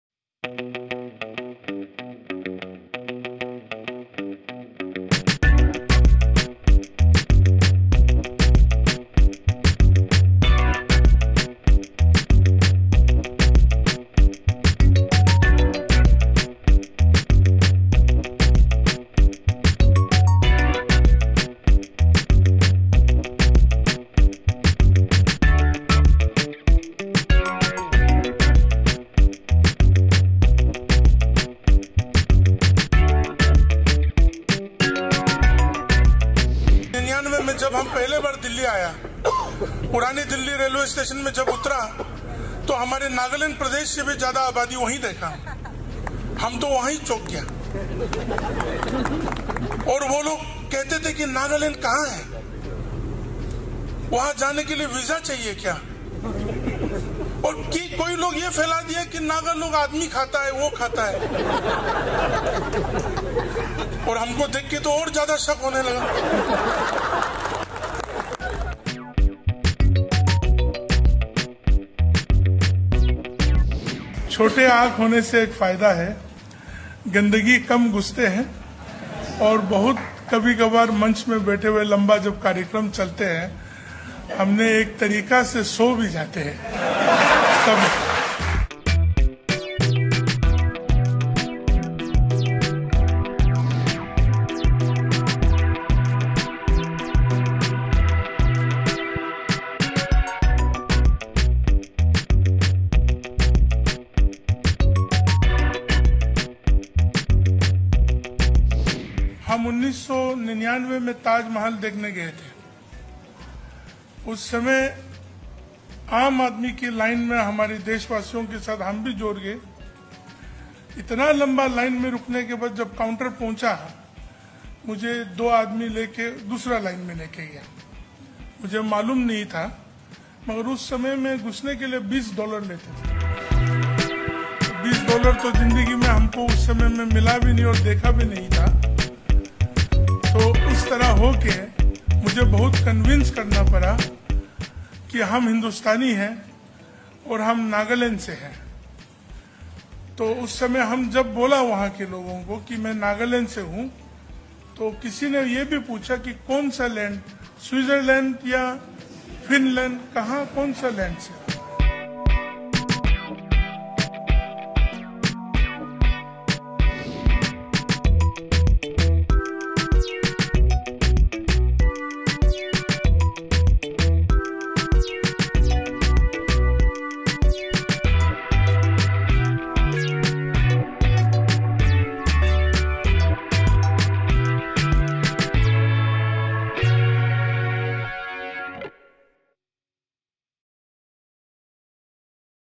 Nagaland Minister Temjen Imna Along's videos are going viral and with good reason
BJP leader Temjen Imna Along has left the internet in splits after he shared stories of his first visit to Delhi. The Nagaland minister said when he first reached Delhi in 1999, it looked like the railway station had more people than his state. He recently went viral with his speech on having. The first instance was caught on video at an event where he was seen sharing the benefits of having "small eyes" and said that he could doze off to sleep during boring events. Here's a compilation of his funny moments.